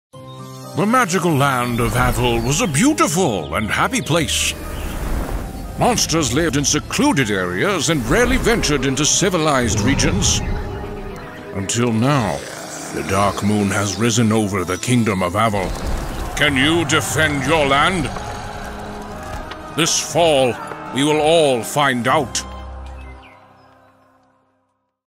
Le Cronache di Avel - teaser
Eroi ed eroine, manca solo una settimana all'uscita di Le Cronache di Avel!
Avel, terra di incredibili luoghi e creature, circondata da decine di magiche lune ha bisogno di voi!
Le Cronache di Avel, dal 14 dicembre!